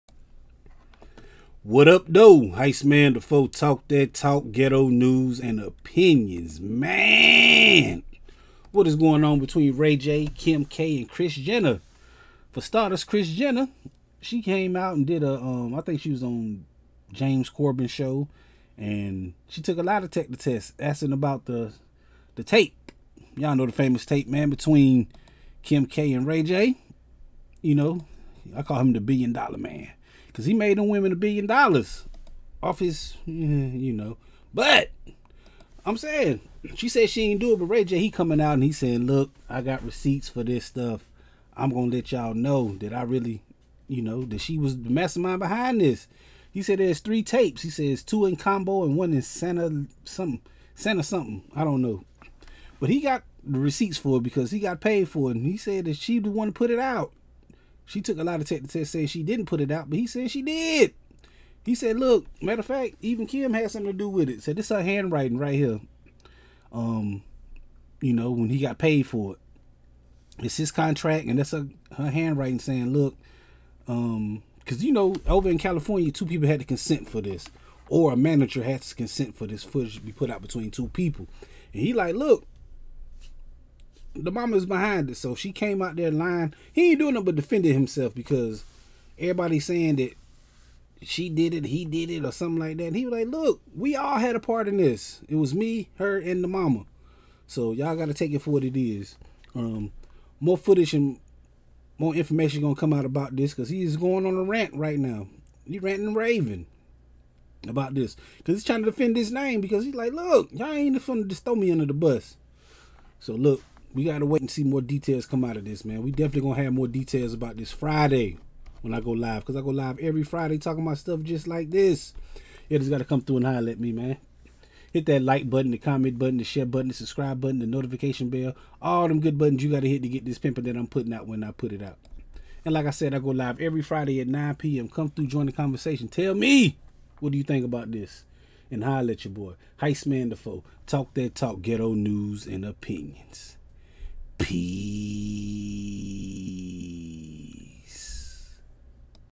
Ray J EXPOSES Kim Kardashian & Kris Jenner CONTRACTS About "TAPE"
Kris Jenner took a lie detector test and said she wasn't responsible for the Kim K and Ray J tape...Ray J calls cap and shows receipts
Maaaaaaan!!!